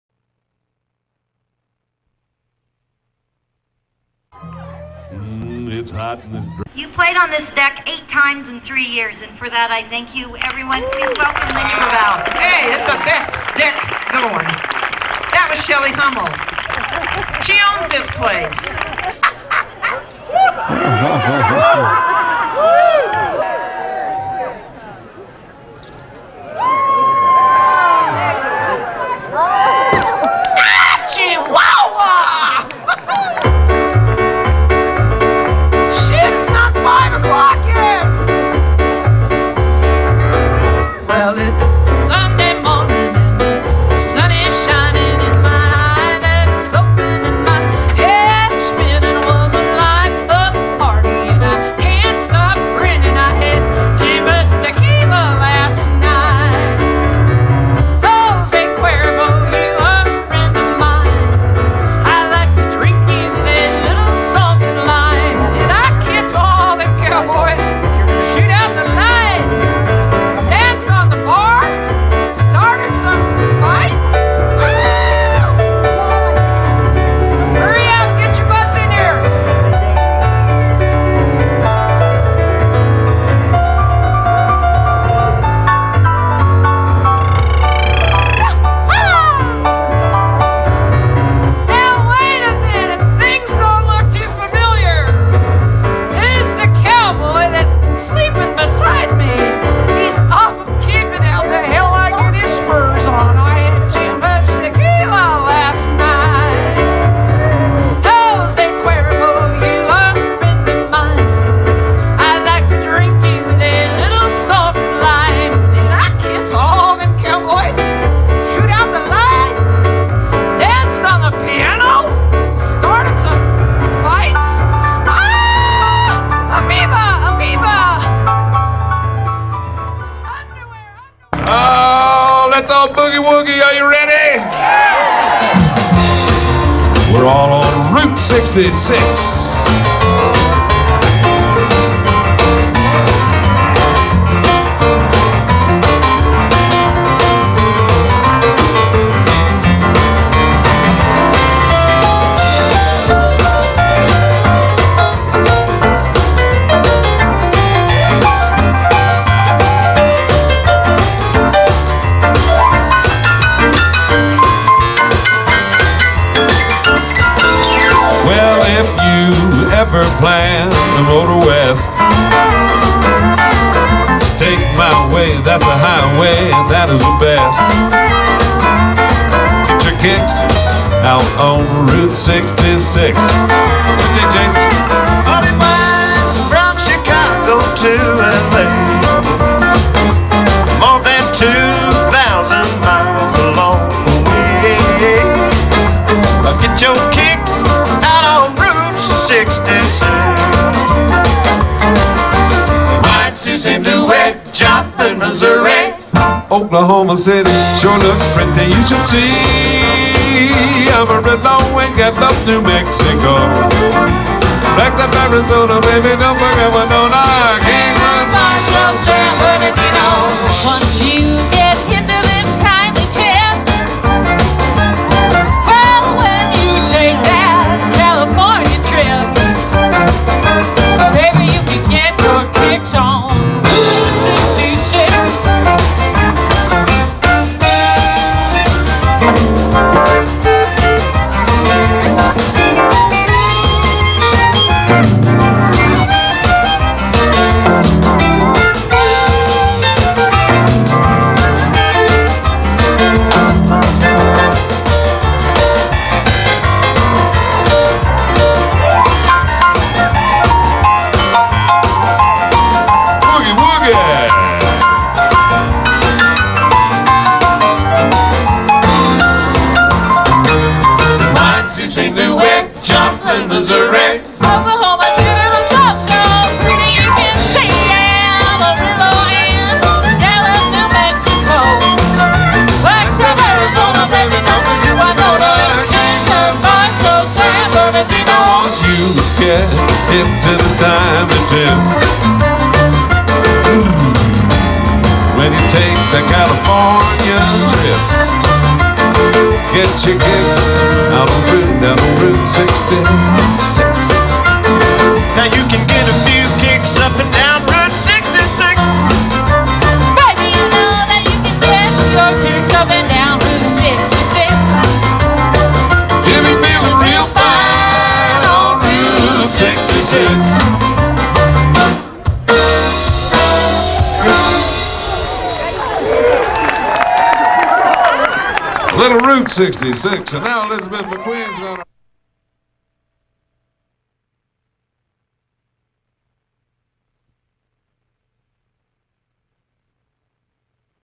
Asleep at the Wheel Benefit Concert
A benefit concert produced by Studio 404 whose proceeds were donated to The Humane Society of Bend, Oregon.